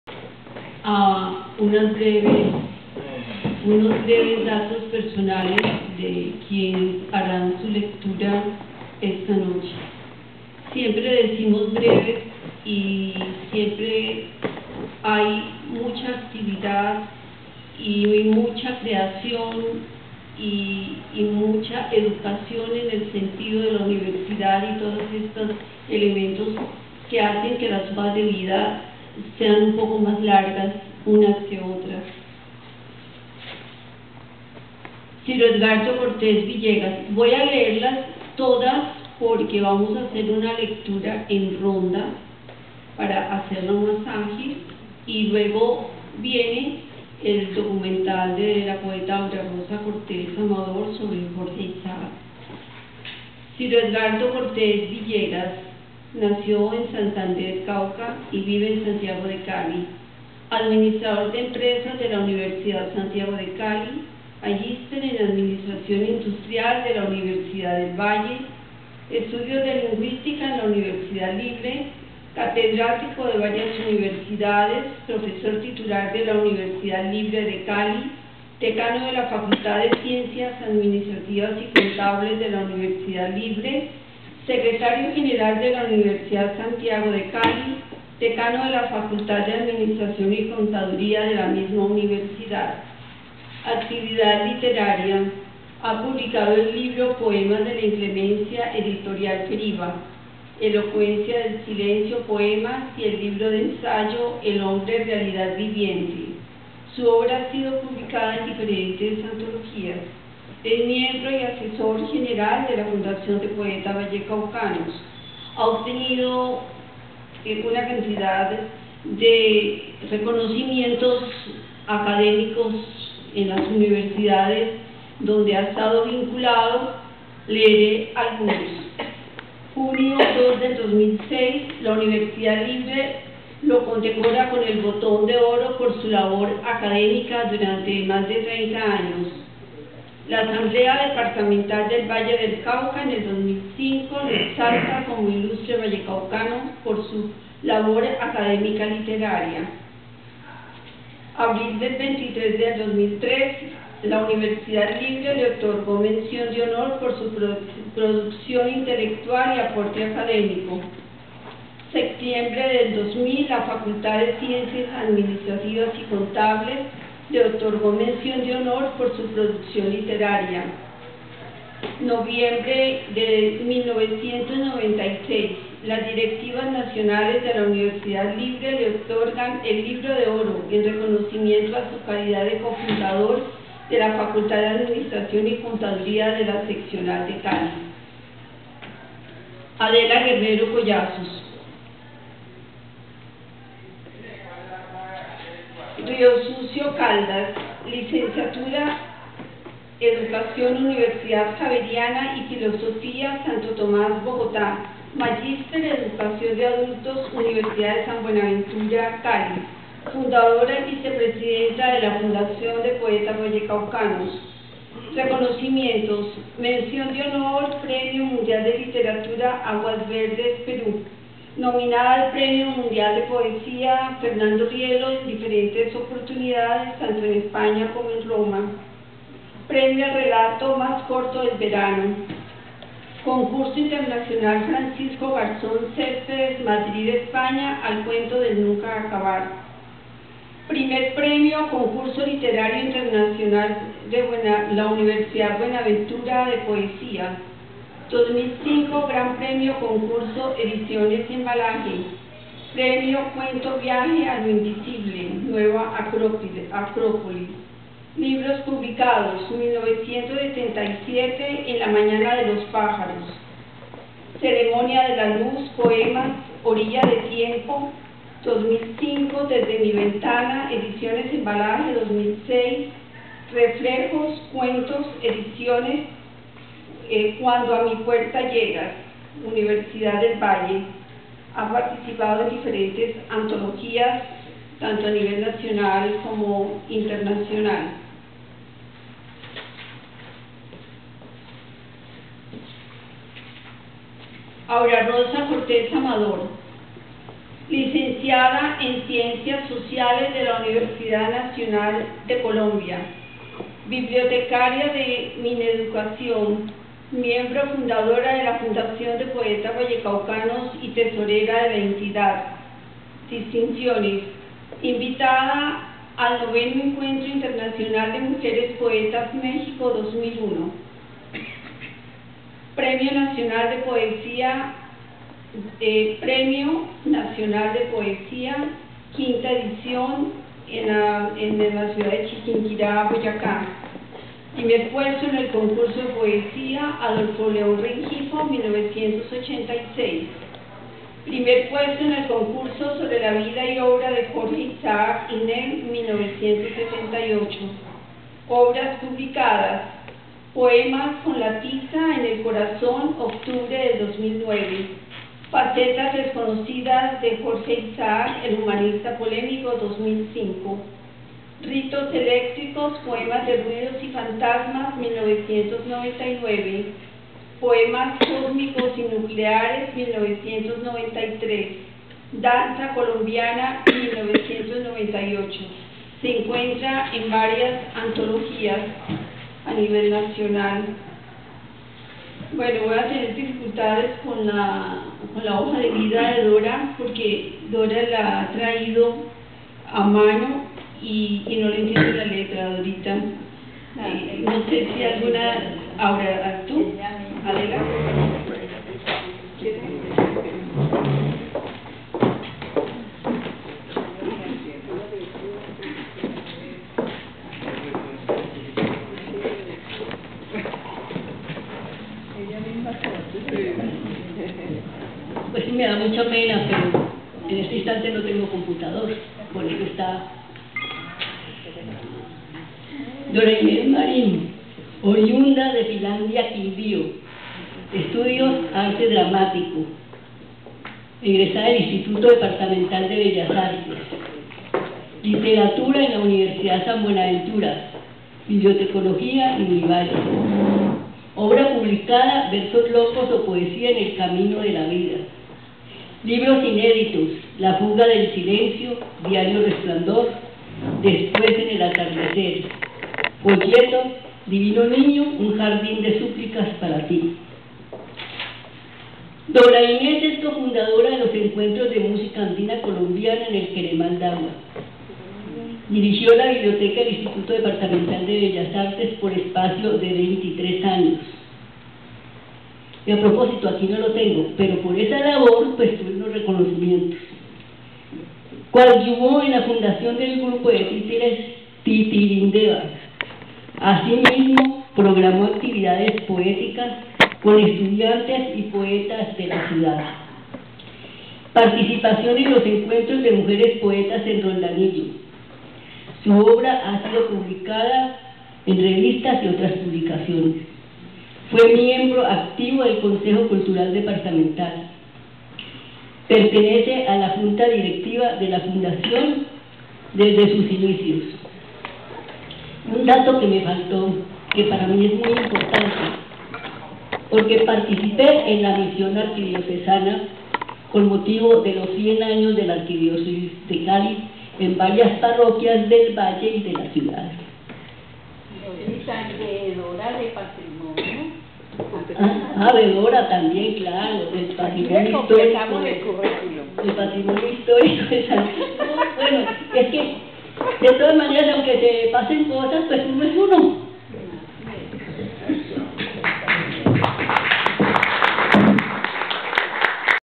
Fundación de Poetas Vallecaucanos. Noche Poética. Nov. 28, 2013. Amparo Romero interviene 2 de 8
*** 28 de Noviembre, 2013, Cali, 6:00 PM. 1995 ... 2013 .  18 AÑOS
--- Fundación de Poetas Vallecaucanos. 18 años. * LECTURAS CONMEMORATIVAS, Celebración.  Leerán poemas los miembros de la Directiva de la Organización: Aura Rosa Cortes Amador, Dora Inés Marín Cifuentes, Adela Guerrero Collazos y Ciro Edgardo Cortes Villegas Y Amparo Romero Vásquez.  Y se presentará el documental "Jorge Isaacs, imágenes del humanista apasionado" realizado por Aura Rosa Cortés Amador.  Lugar: Centro Cultural Comfandi, Calle 8 No. 6-23, piso 3. Copa de vino. Entrada libre.